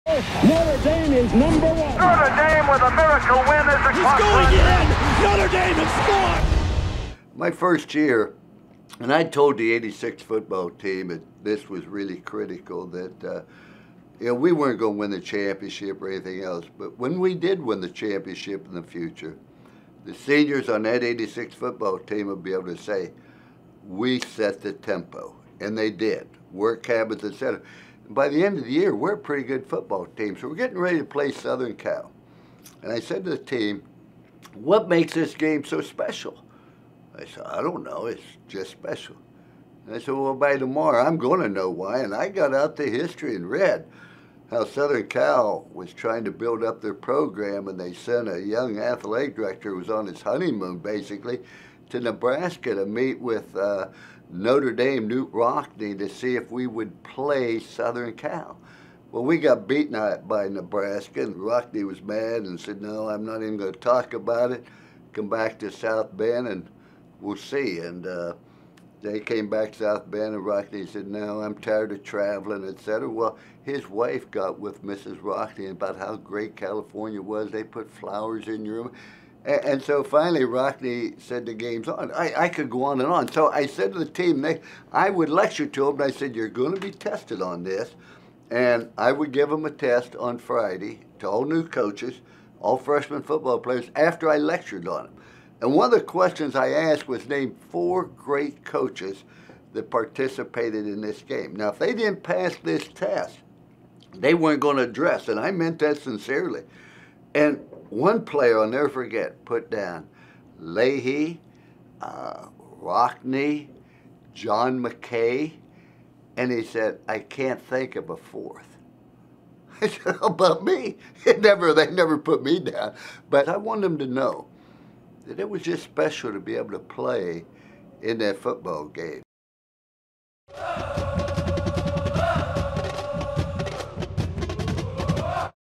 Lou Holtz's USC Test - 125 Years of Notre Dame Football - Moment #087
In a recent interview with Fighting Irish Digital Media, Notre Dame coaching great Lou Holtz talks about the history of the USC rivalry and the test he used to give to his new players and coaches before the annual game against the Trojans.